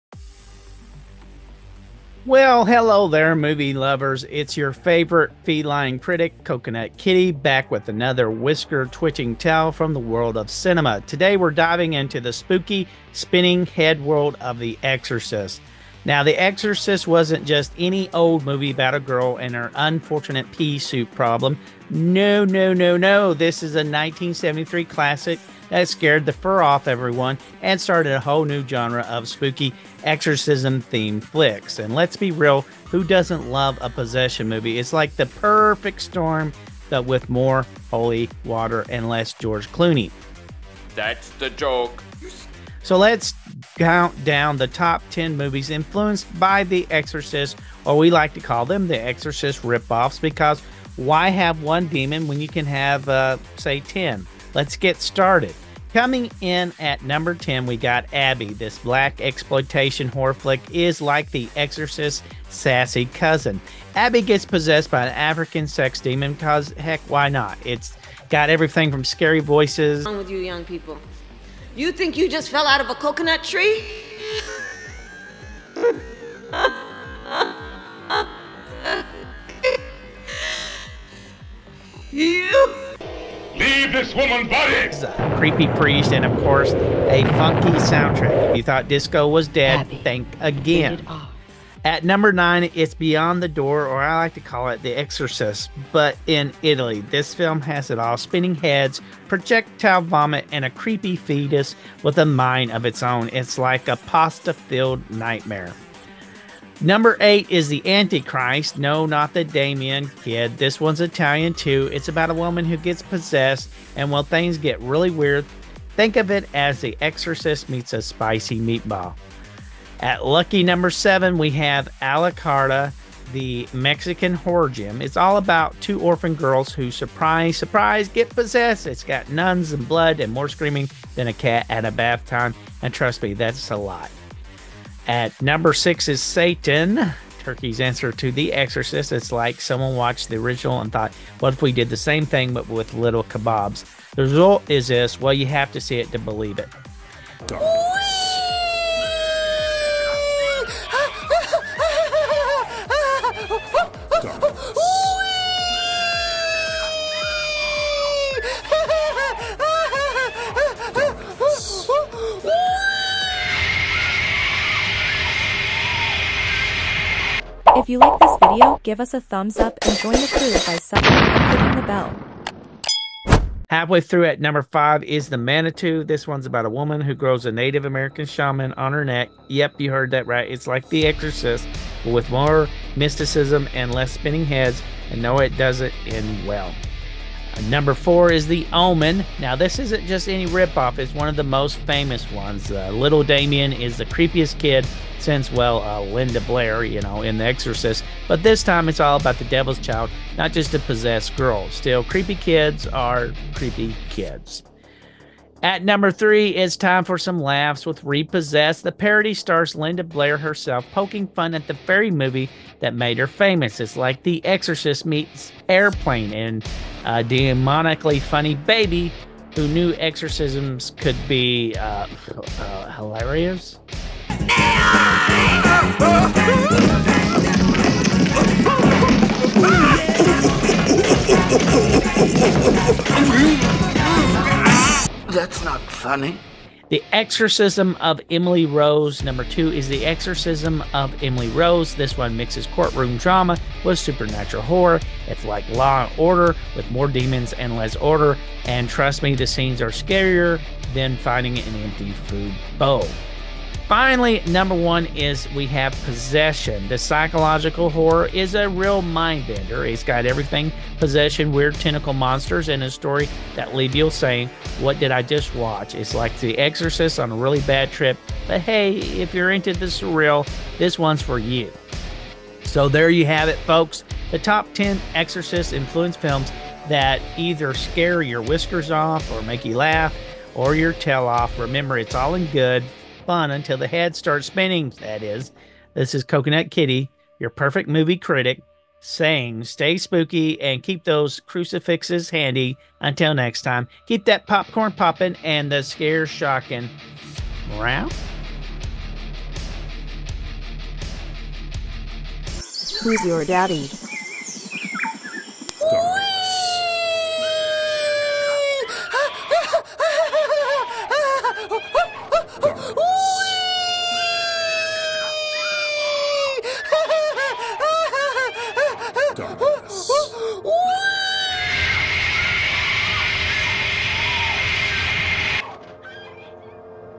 The Feline Movie Critic on The Exorcist and Its Influences
Read More Funny Stories Here!
"Well, hello there, movie lovers! It's your favorite feline critic, Coconut Kitty, back with another whisker-twitching tale from the world of cinema. Today, we're diving into the spooky, spinning-head world of The Exorcist! 🎬👻"
Our Discord to discuss Movies and Pictures